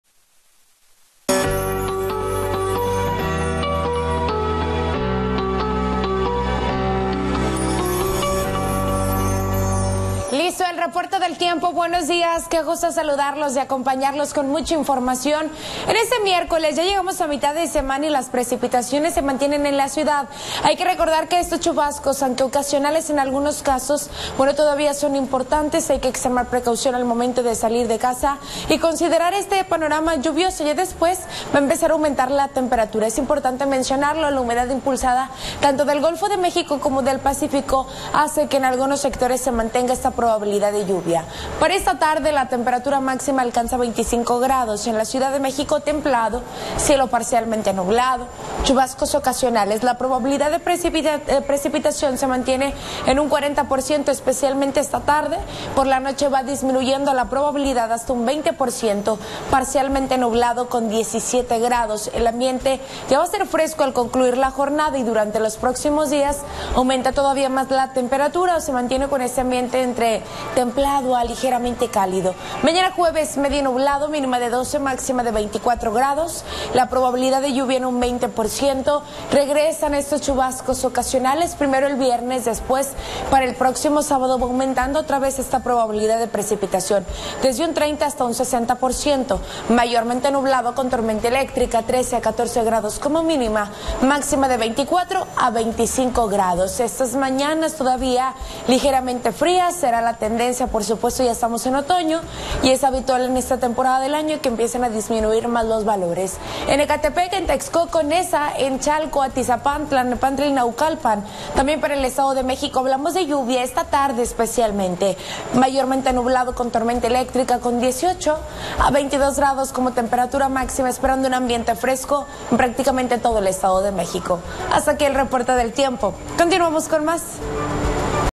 Clima 11 de octubre con Pamela Longoria
¡Buenos días! Tenemos 40% de probabilidad de lluvia, parcialmente nublado y chubasco ocasional en la CDMX 11 de octubre del 2023.